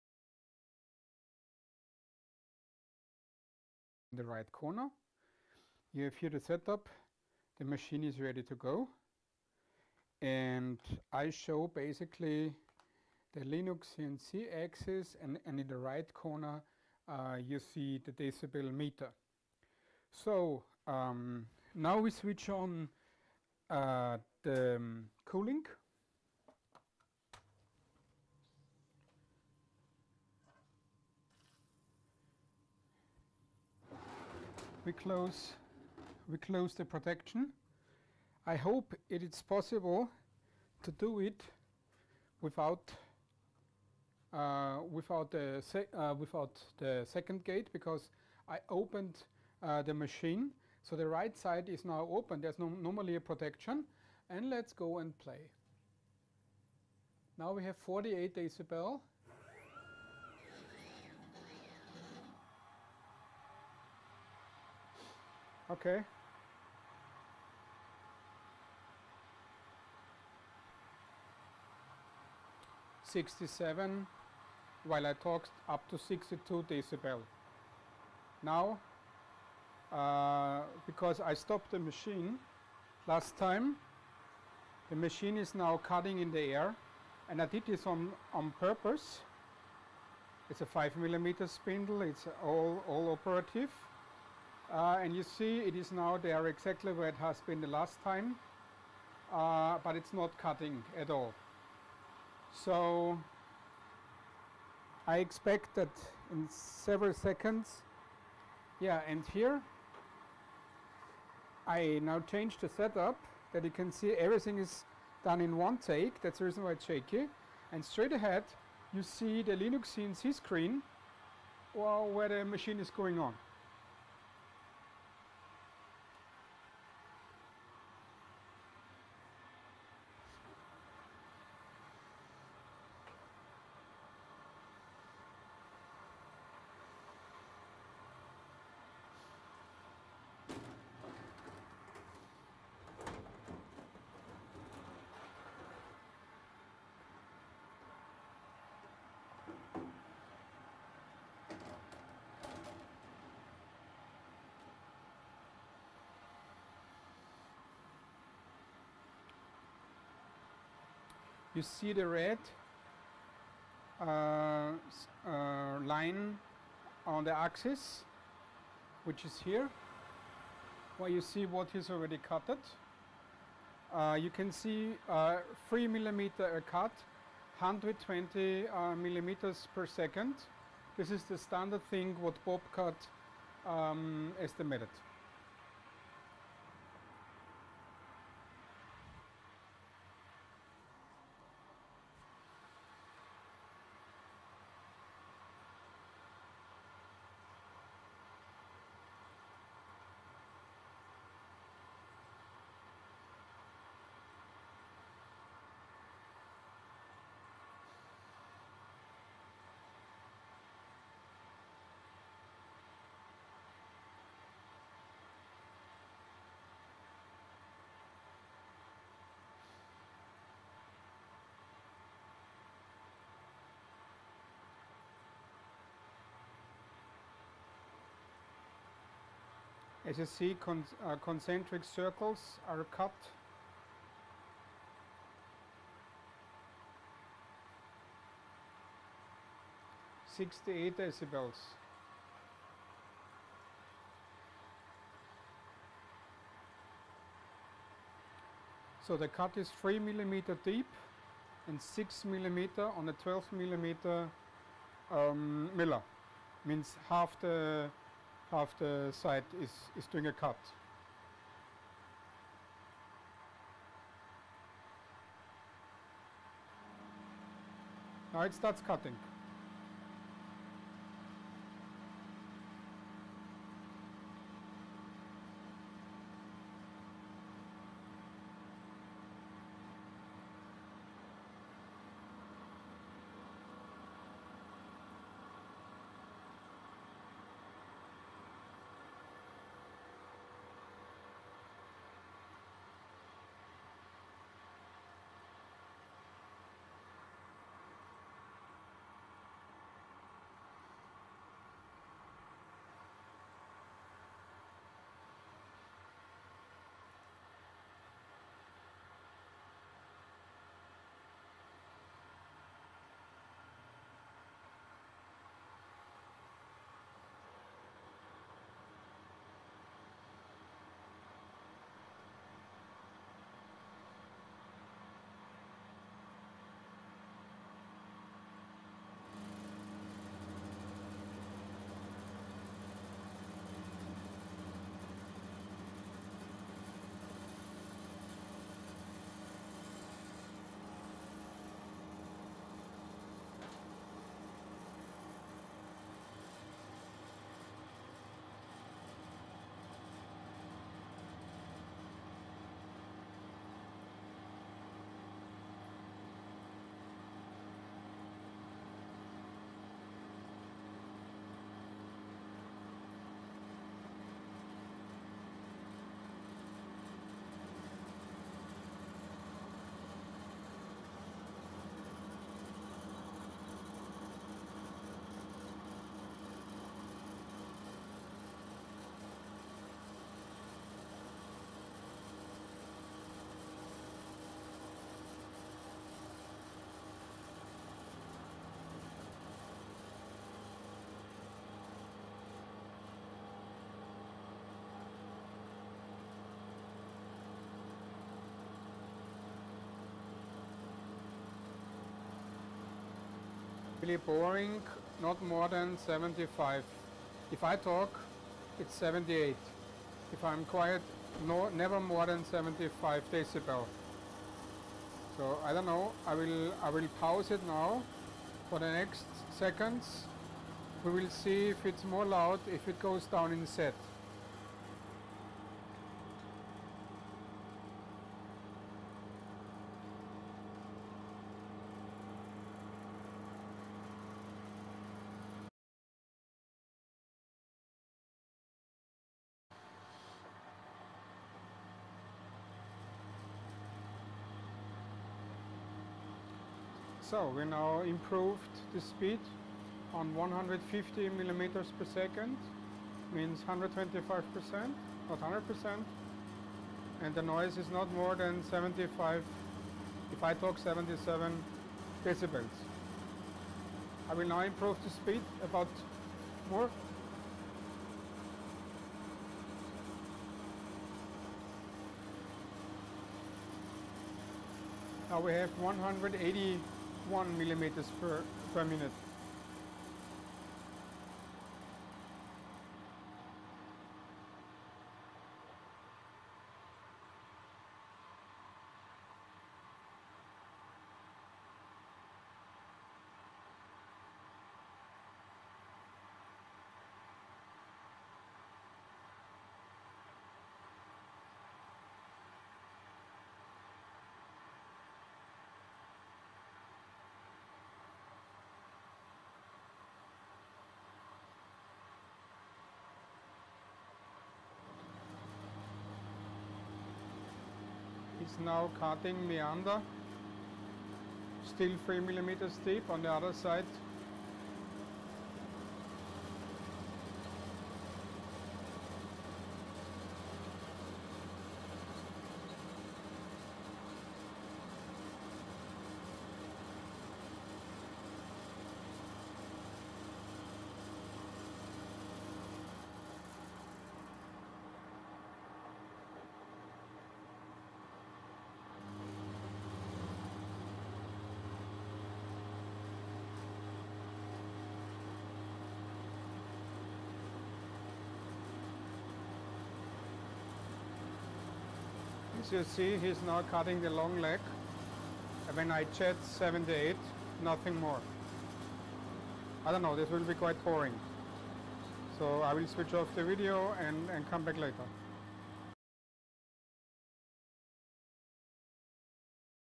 Thojomech TFC11 what about noise ? Part 3 The cut in Alu + higher speed
we exchanged the flood cooling so now we proceeded with cutting alu 12mm 4 flute miller 3 mm deep and 6mm cuts (half the miller ) the starting speed was 120mm/min which we increased up to 180mm